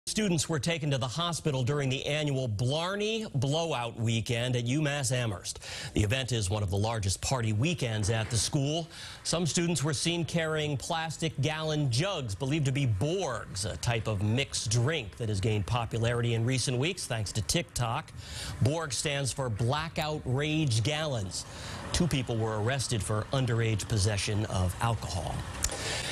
Blarney Blowout and borgs send dozens to the hospital
The annual party at UMass Amherst saw dozens taken to the hospital and two arrested for underaged possession of alcohol.